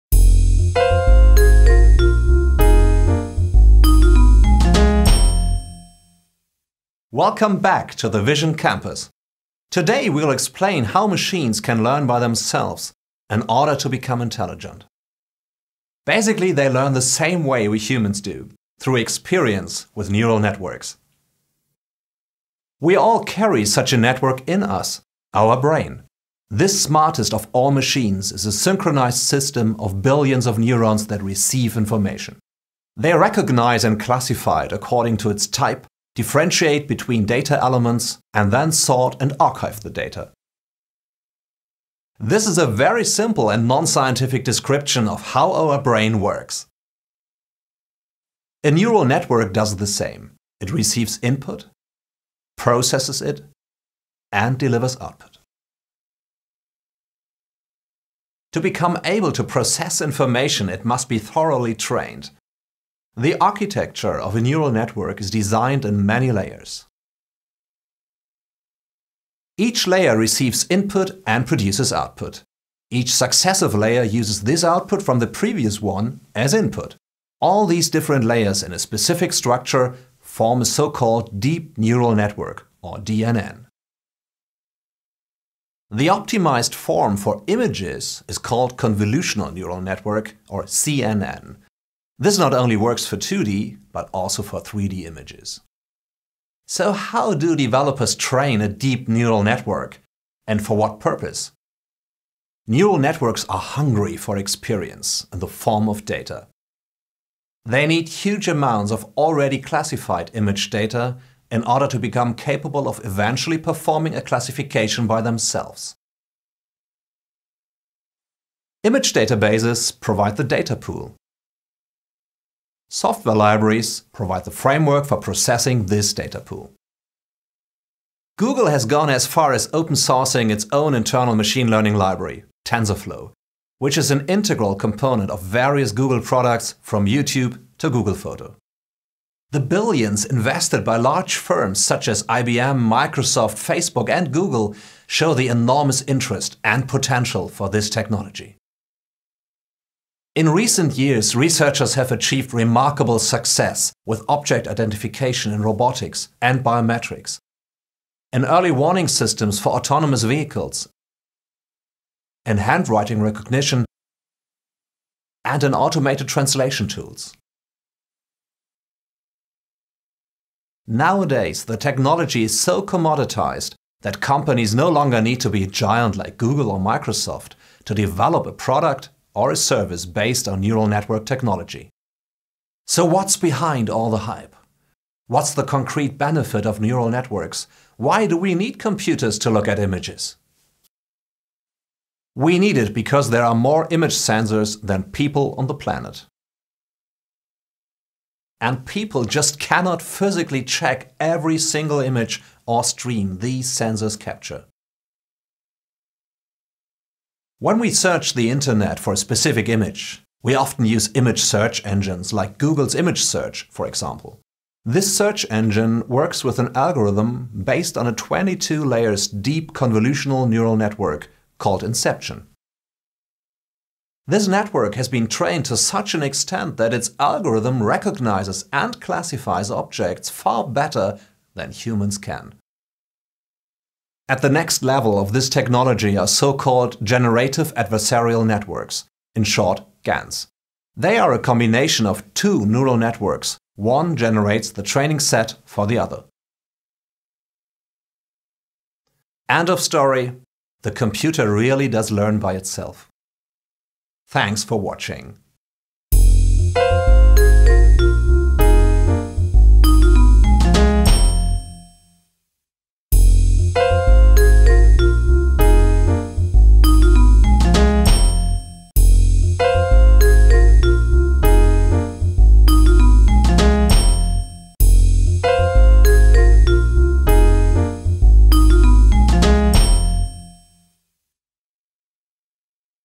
How Do Machines Learn? – Vision Campus
How do we teach machines to learn by themselves and eventually become “intelligent”? With neural networks just like the one we all carry in us – the brain.
Machines learn by experience and with the help of neural networks. Just like in the human brain, a network of billions of neurons receives and recognizes information, classifies, sorts and archives it. A neural network basically works the same way: it receives input, processes it, and delivers output.
Watch our Vision Campus video to learn about the different types of neural networks, how they are designed, and how their algorithms operate everyday functions such as image search, product recommendations, autonomous vehicles, or automated translation tools.

00:10 How can machines become intelligent?
00:26 The human brain – the smartest of all machines
00:52 How does a neural network work?
01:04 The architecture of a neural network
01:19 Deep neural network (DNN) and convolutional neural network (CNN)
01:38 How to train a DNN
02:00 Data pool and framework
02:32 Object identification based on neural networks
03:05 The benefit of neural networks
03:10 Why do we need computers to look at images?
03:59 The next level: generative adversarial networks (GANs)
04:13 Conclusion

to find all of our exciting videos and articles about vision technology. Just make sure you stop by!